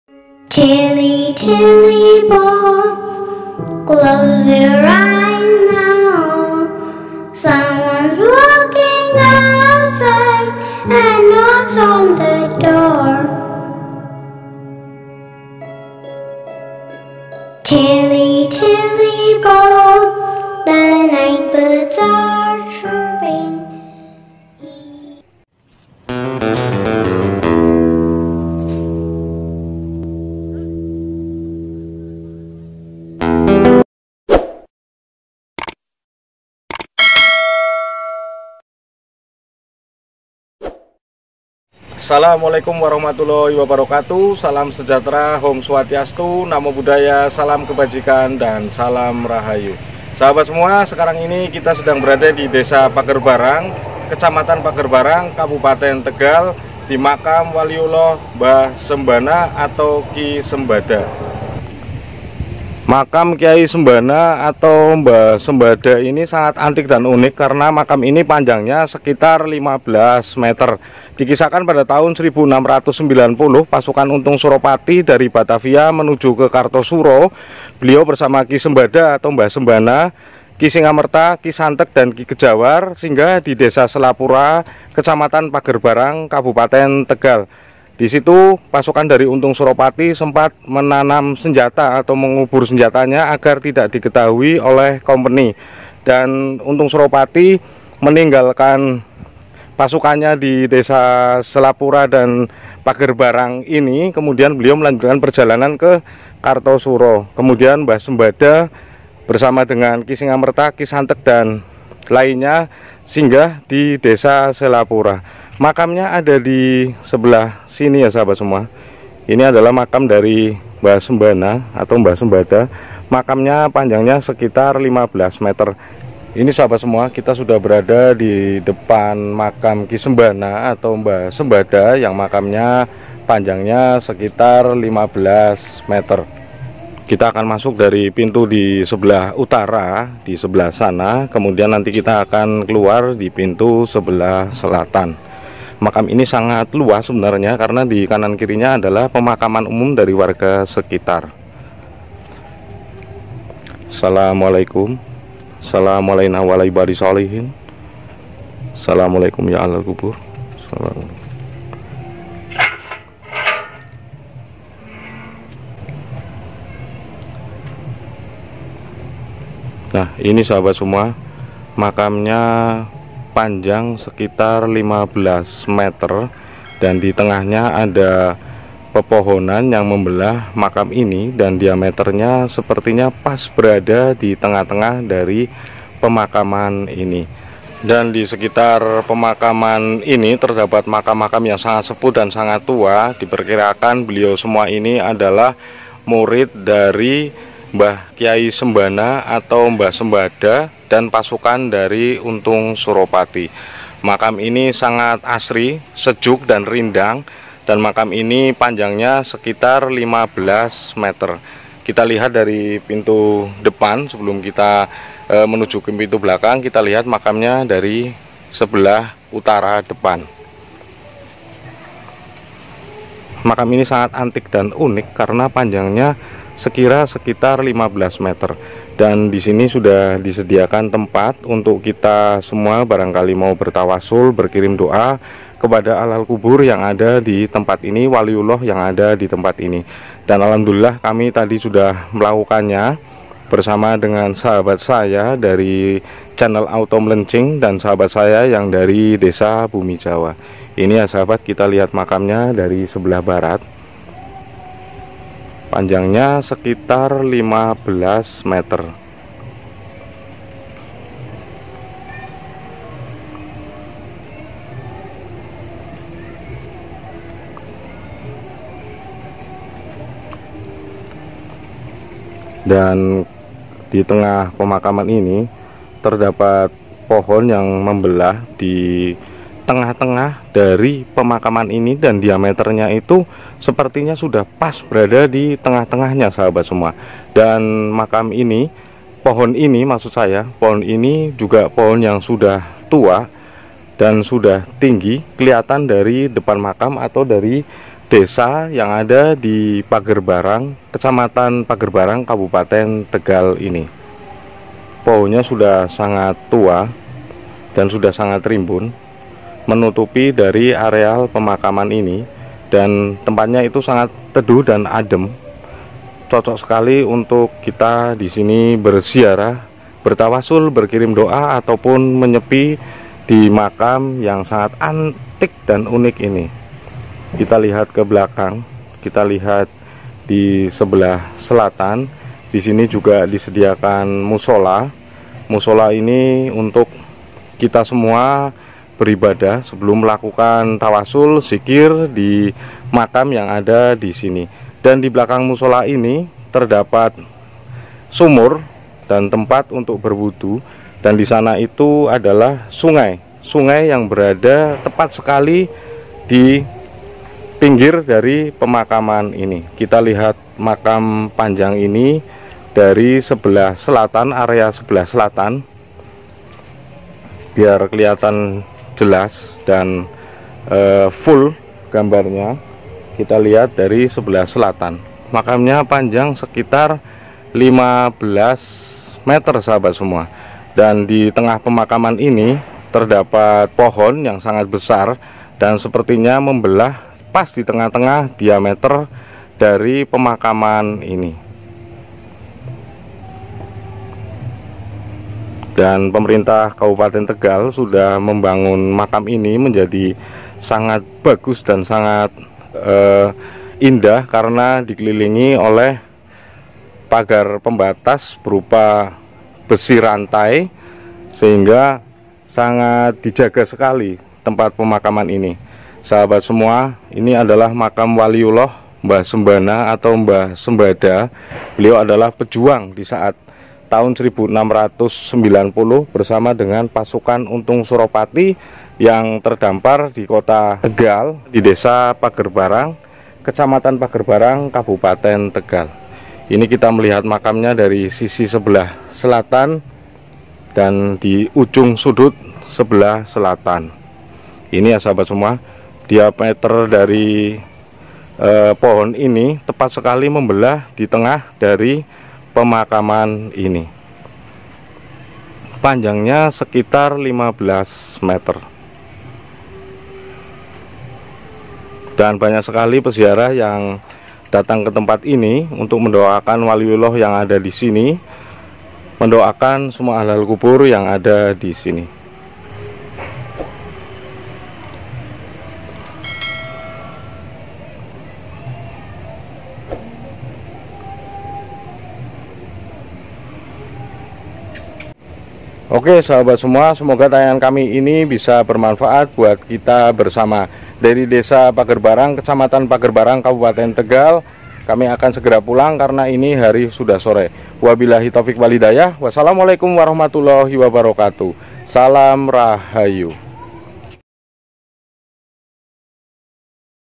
MAKAM DOWO‼️MAKAM KI SEMBANA
Makam Dowo atau Makam Panjang ini terletak di desa Pagerbarang Kecamatan Pagerbarang Kabulaten Tegal Jawa Tengah.

Paseban Kyai Sembana atau Mbah Sembada ini tidak Lepas dari Kisah Perjuangan Untung Surapati yang di Kejar oleh Kompeni/Belanda dari Batavia Menuju Kartasura Jawa Tegah.

Untung Surapati Sempat Singgah di Desa Kalisoka Kecamatan Dukuhwaru Kabupaten Tegal Jawa Tengah Untuk Bertemu dengan Adipati Tegal Rekaonegoro 1,Sedangkan Ki Sembana,Ki Santeg,Ki Singamerta dan Ki Kejawar Menetap di desa Pagerbarang Kecamatan Pagerbarang Kabupaten Tegal Jawa Tengah.